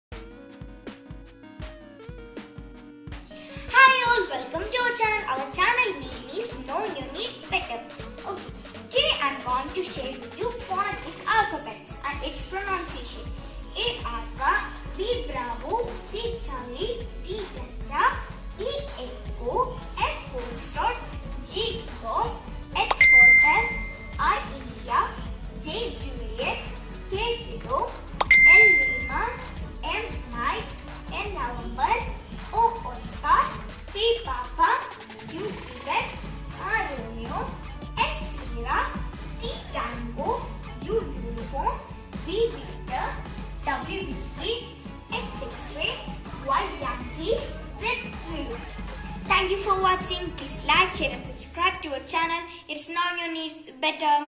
Phonetic Alphabet | The International code | Mainly used by Police and army officers | Must watch
A Alpha
B Bravo
C Charlie
D Delta
E Echo
F Foxtrot
G Golf
H Hotel
I India
J Juliet
K Kilo
L Lima
M Mike
N November
O Oscar
P Papa
Q Quebec
R Romeo
S Sierra
T Tango
U Uniform
V Victor
W Whisky
X X-Ray
Y Yankee
Z Zulu
This video is only for learning purpose.